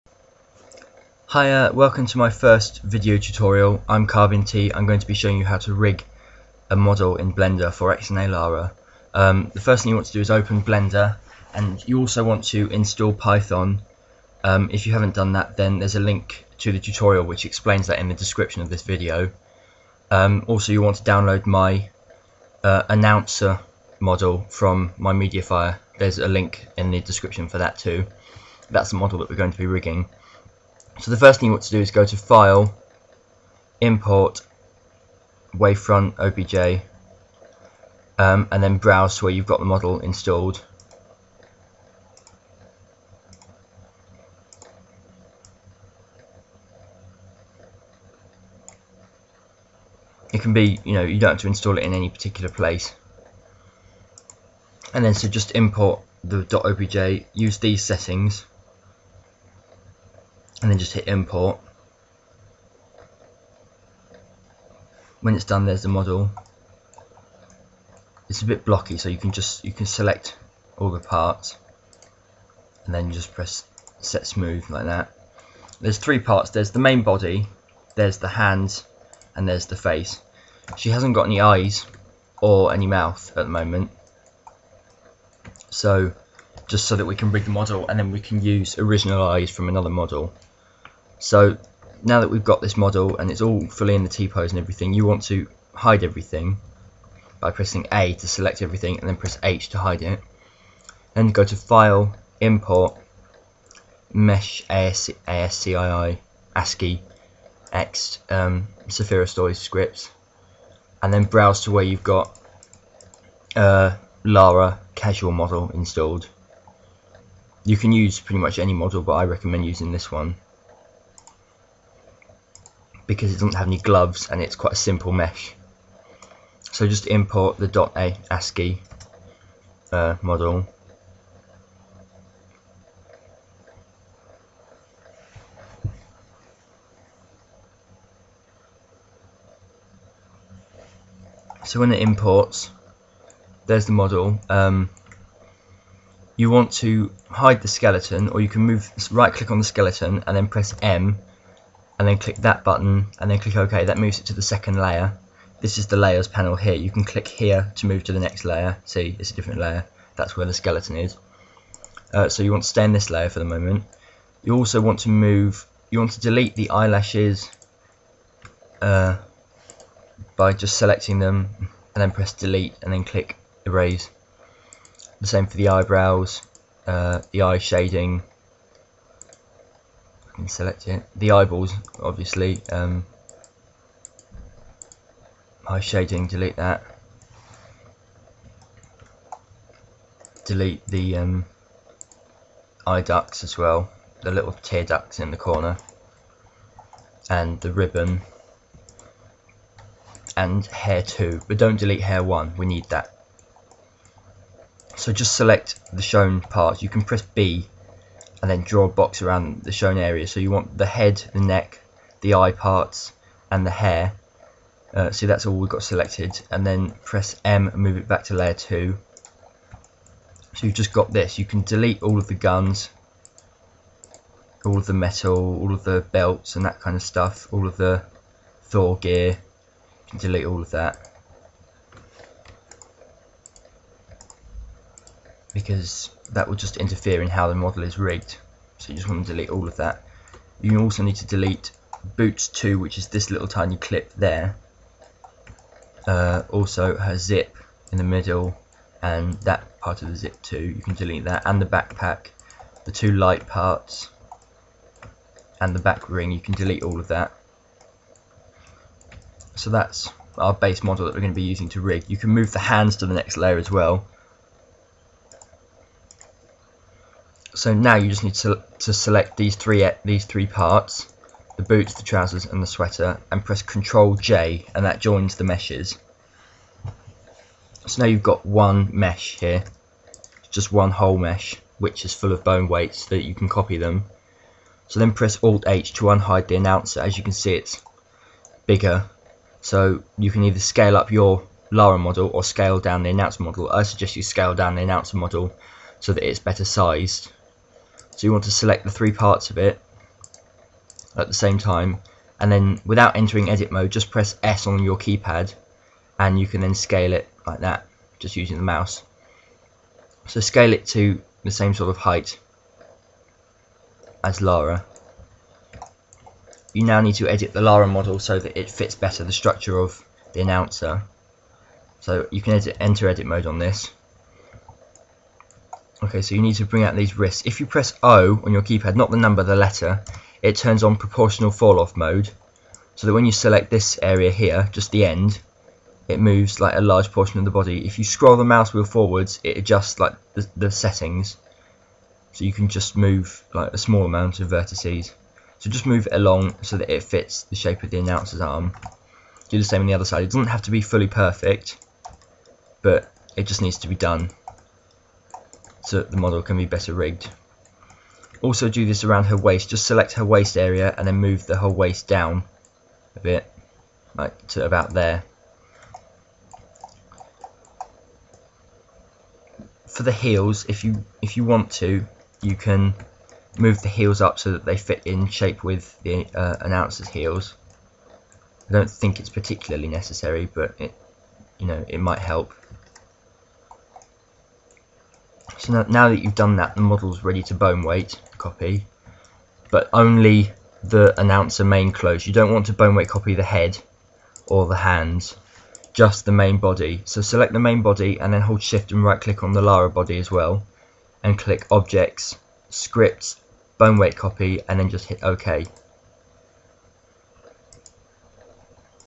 Full Model Rigging For XNALara In Blender Part 1
Part 1 of my mesh-modding tutorial explaining how to rig a model for XNALara.
That's how to install Blender